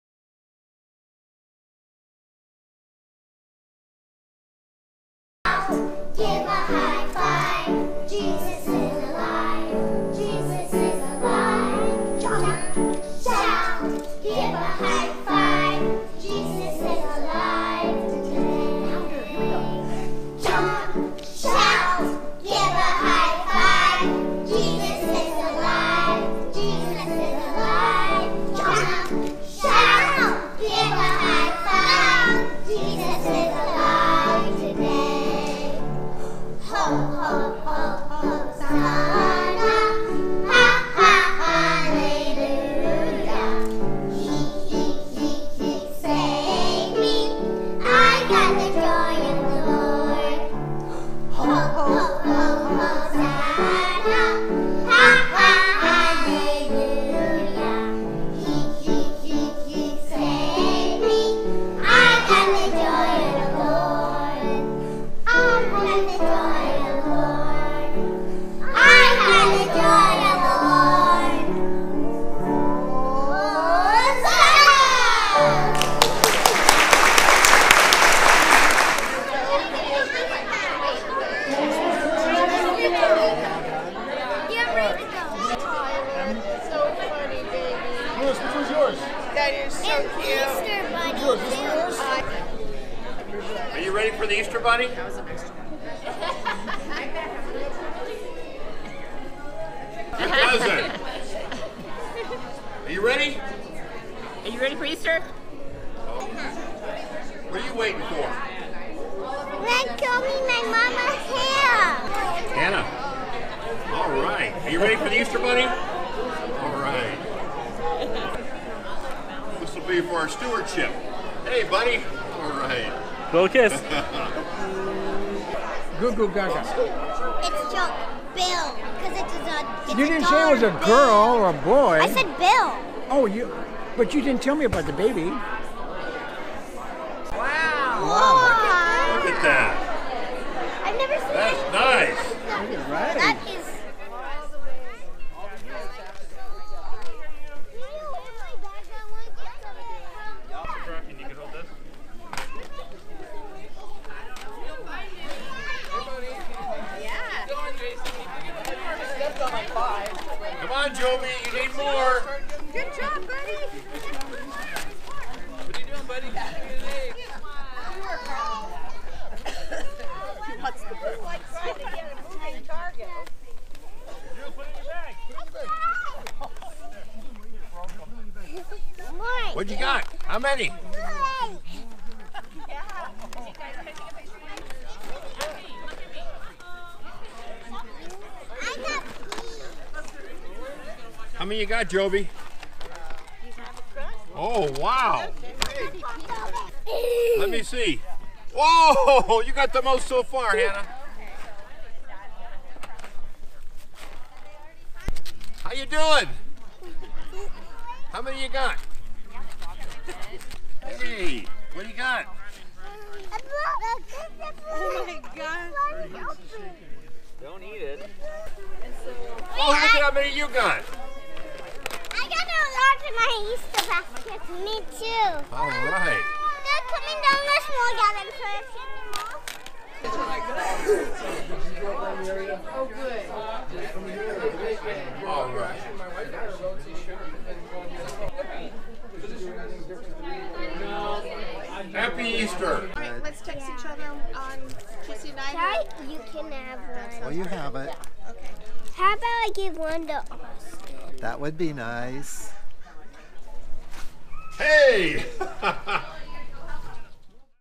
CHPC Easter Egg Hut
Palm Sunday CHPC Easter Egg Hut. Canyon Hills Presbyterian Church in Anaheim Hills, CA.